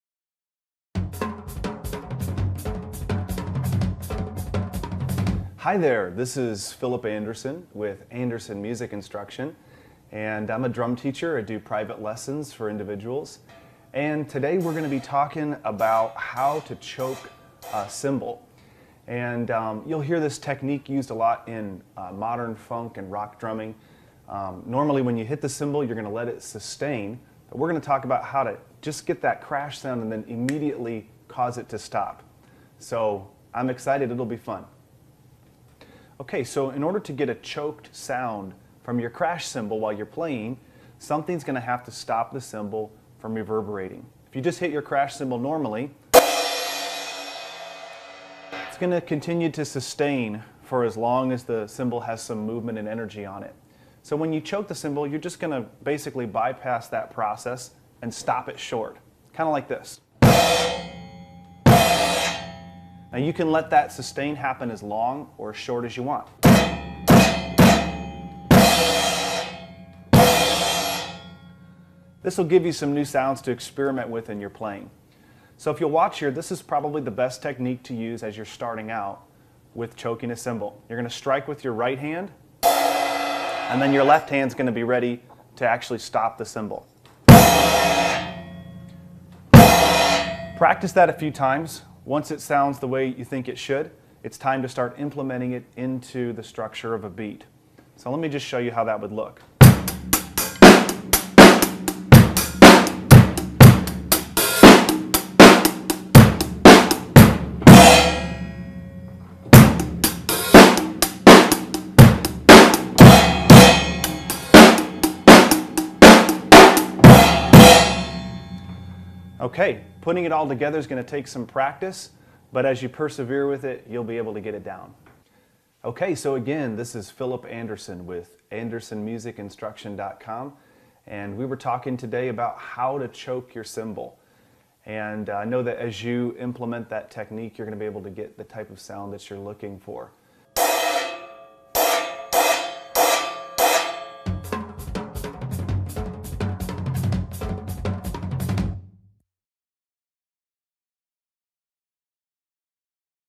How to Choke a Cymbal
How to Choke a Cymbal. Part of the series: Drum Techniques. Choking a cymbal requires you to complete a few very specific steps in rapid succession. Learn how to choke a cymbal with help from a private music instructor in this free video clip.